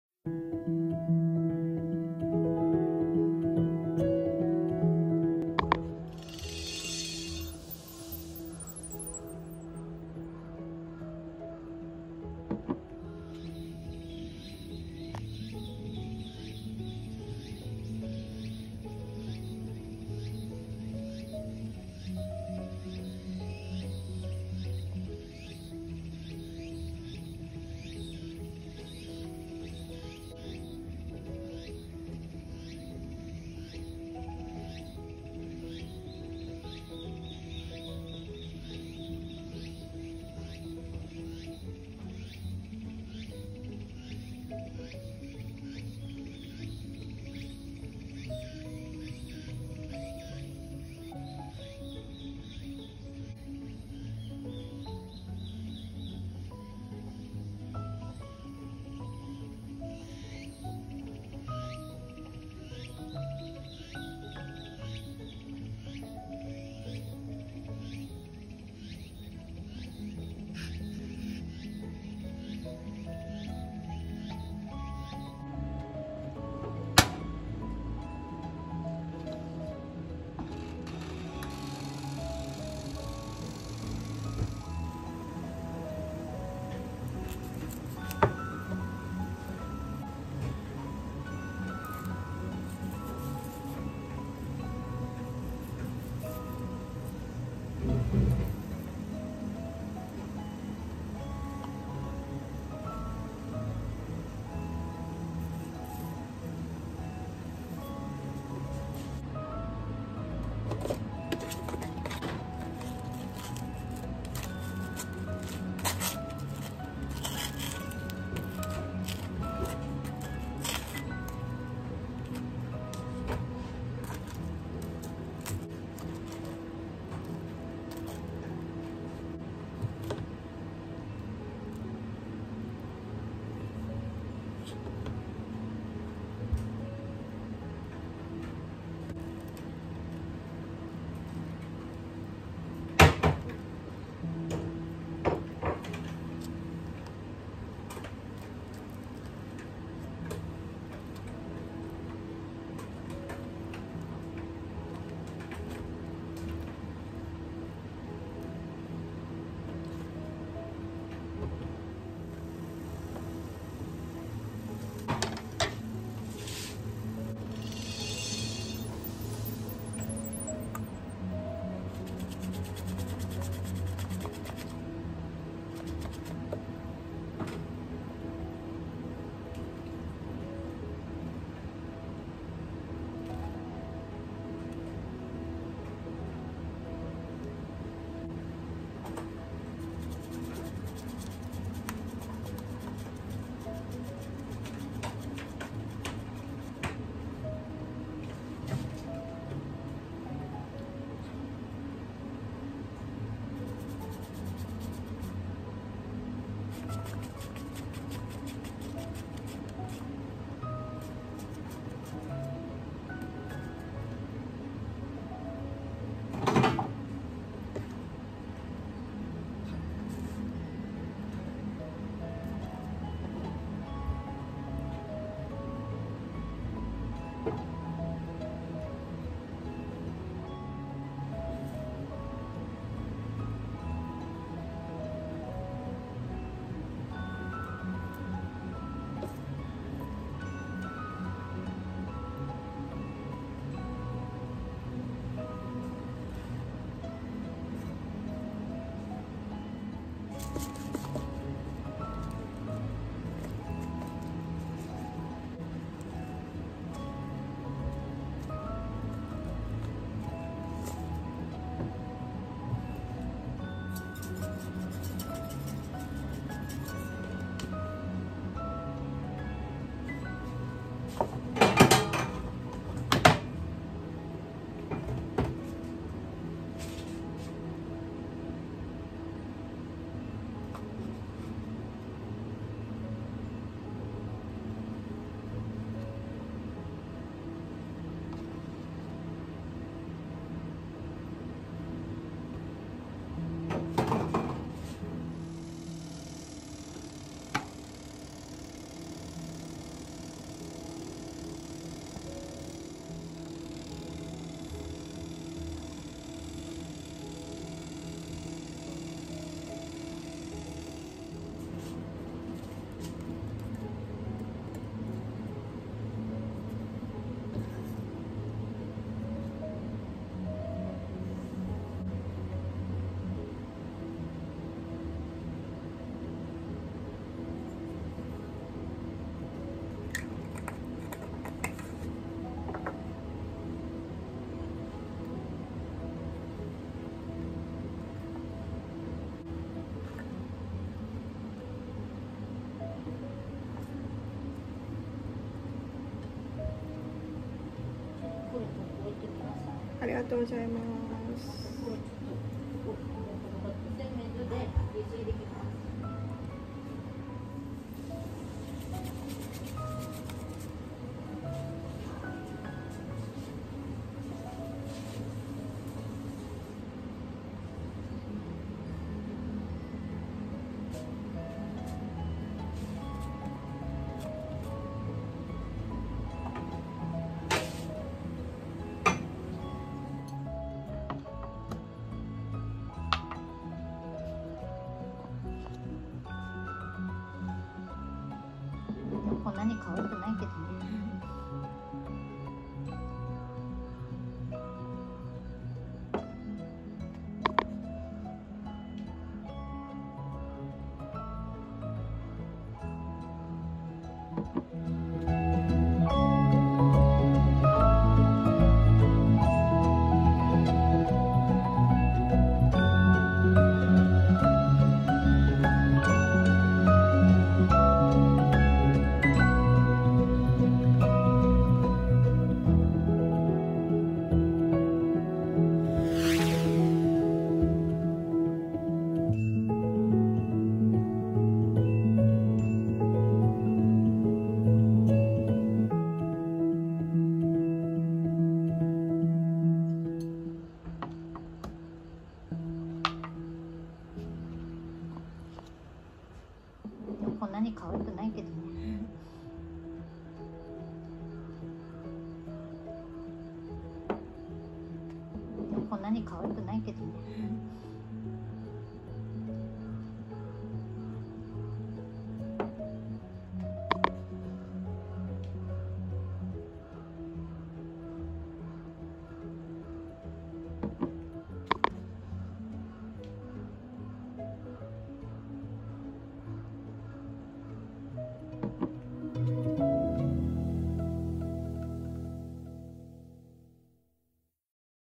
山小屋あけびルーティン
Mushroom Cream Cheese Toastを作りました。
特別のレシピが　あるわけでは　ないのですが
私が　食べて　美味しそうな　料理を作っています
皆様も　是非、お試ししてみては　如何でしょうか。